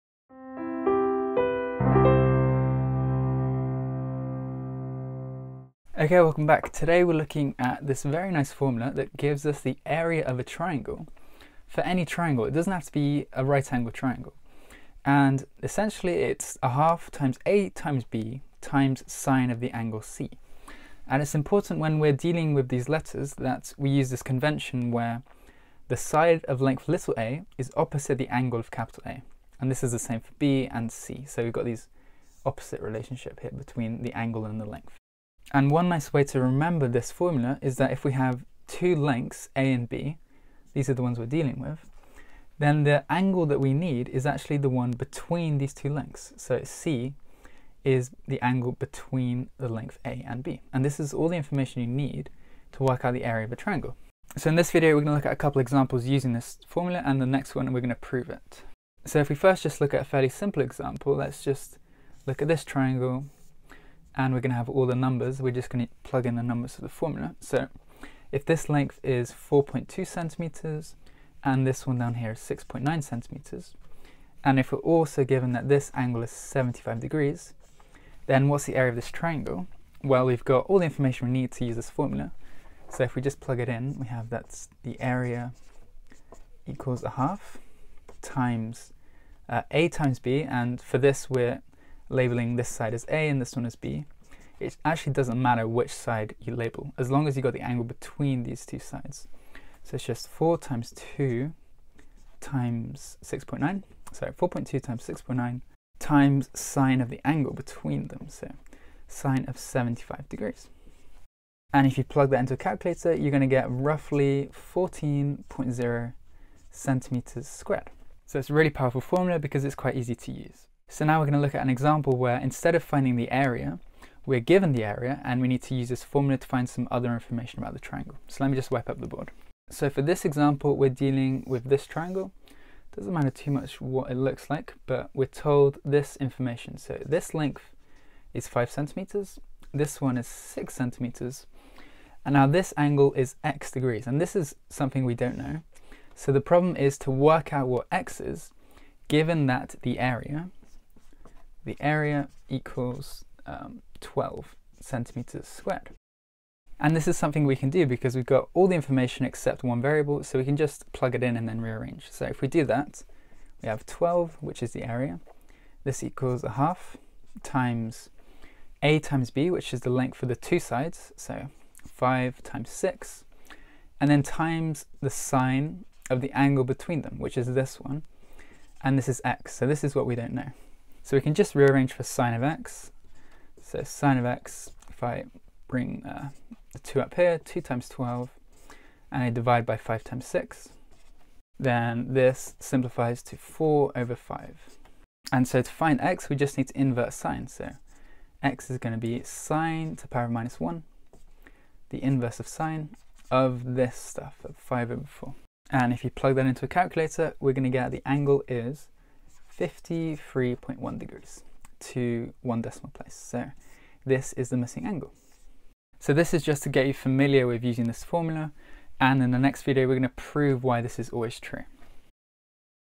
Area of a triangle using sine
How to calculate the area of a triangle using the sin function.